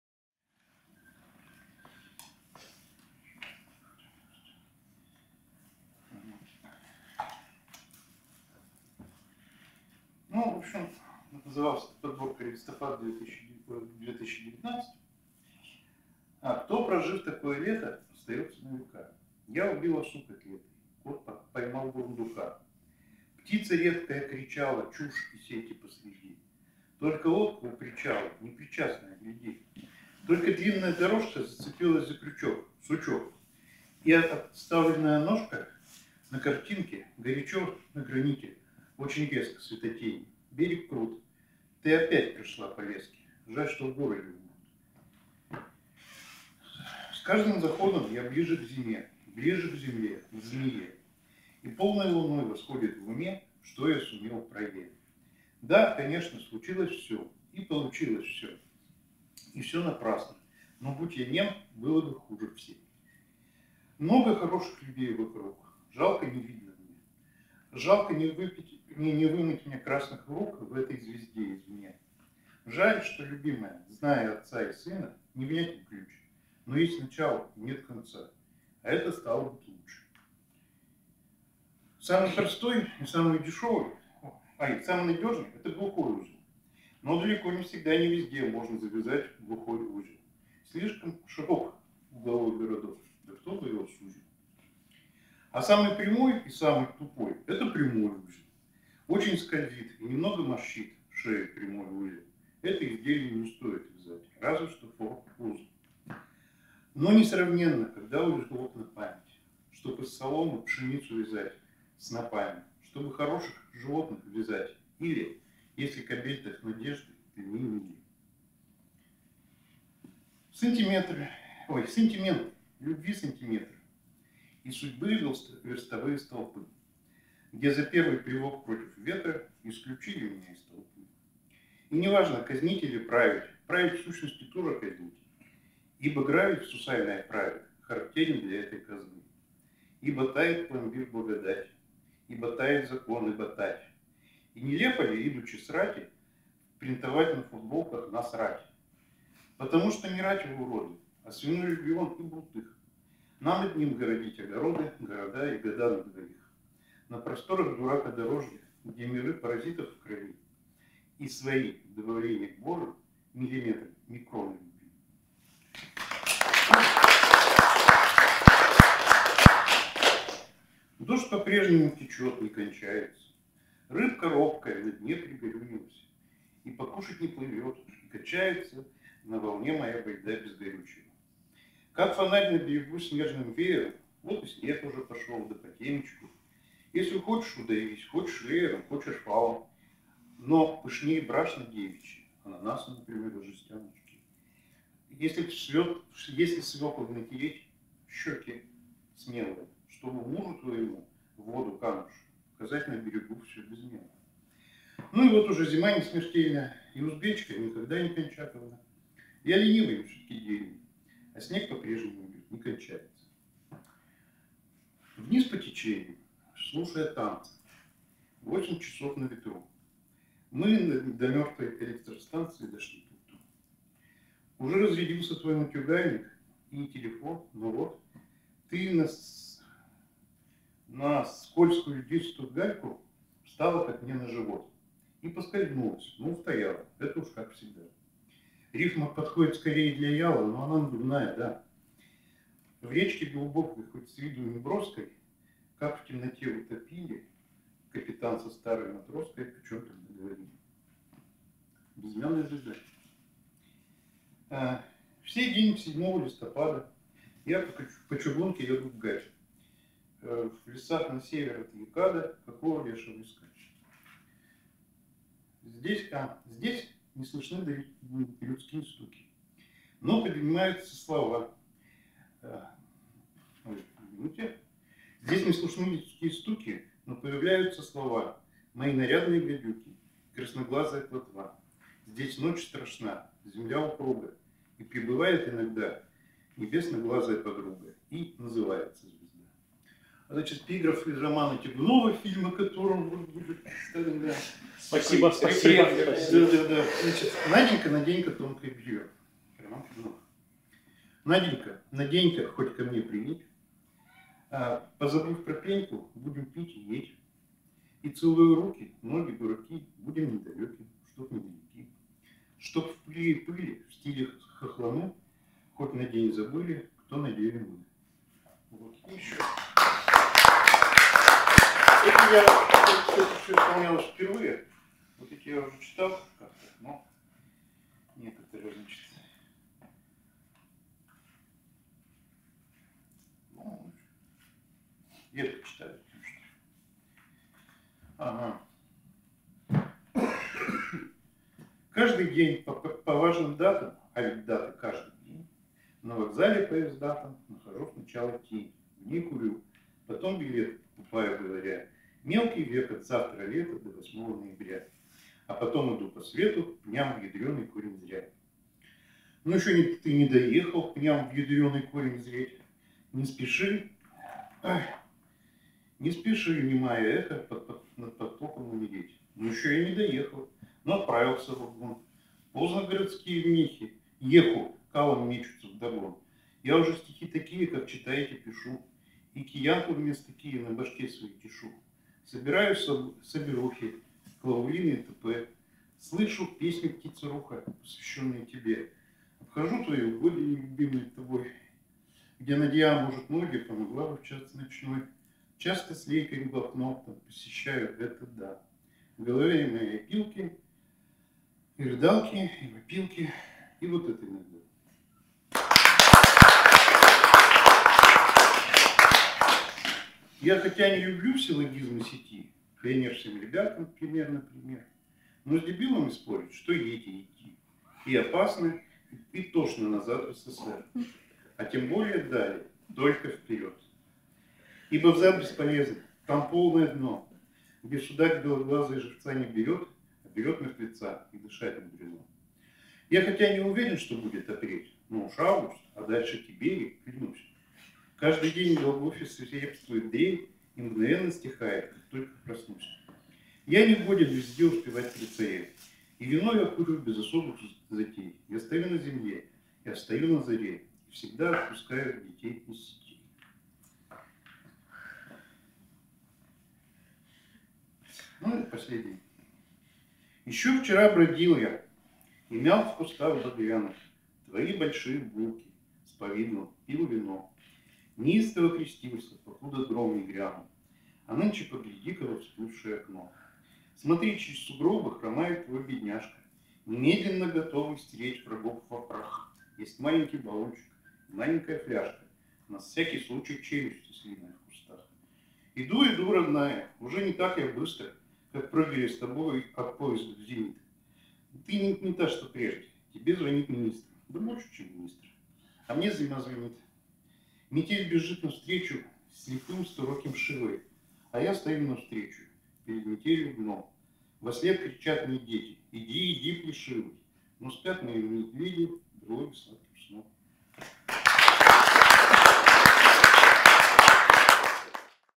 Андрей Ильенков
Андрей Ильенков на поэтическом вечере постапокалиптического позитива «Над пропастью не ржи!».
Екатеринбург, музей «Литературная жизнь Урала XX века», 27 марта 2024 г.